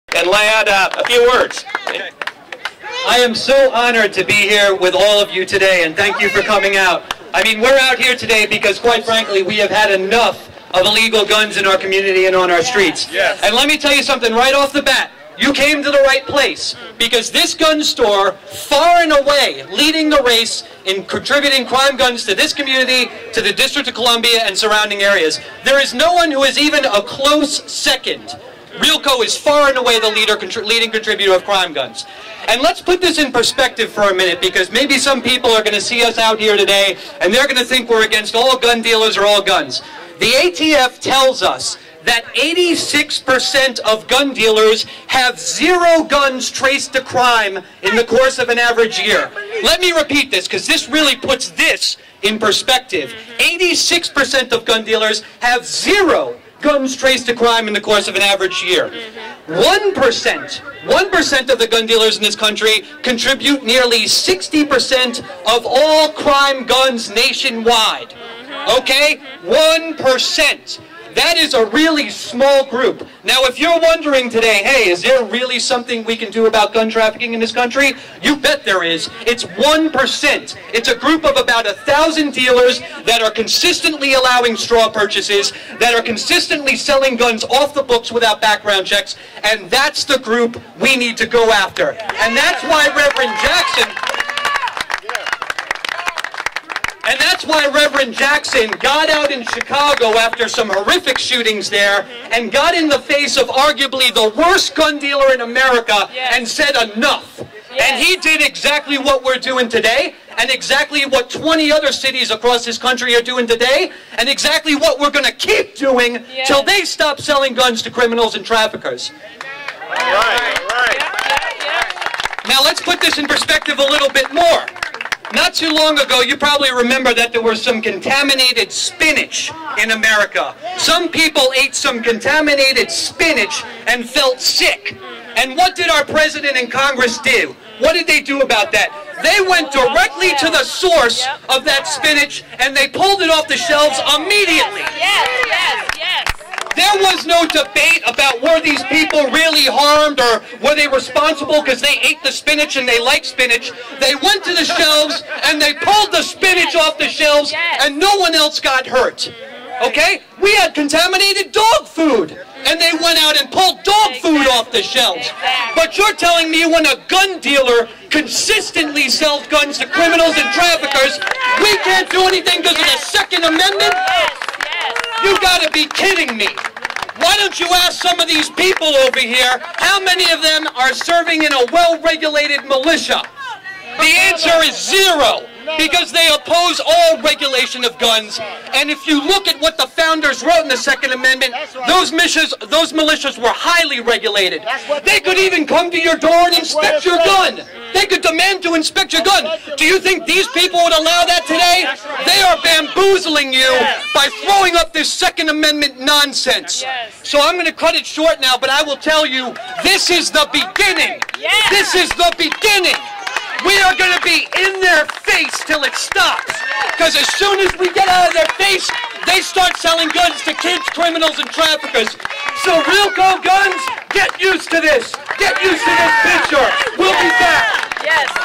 Ladd (MMM) Speaks at National Protest: District Heights, MD
Ladd Everitt, President of the DC Chapter of the Million Mom March, speaks out at the protest in front of Realco Guns.

On August 28th, in more than 25 cities...from District Heights, Maryland and Newark, New Jersey to Seattle, Washington and Dallas, Texas...citizens protested the scourge of illegal gun trafficking in their communities.

The Brady Campaign and its network of Million Mom March Chapters supported Rainbow PUSH Coalition's efforts by organizing local events to call on leaders to stop illegal gun trafficking and keep guns out of the wrong hands. The National Day of Protest marked the anniversary of Dr. Martin Luther King's March on Washington.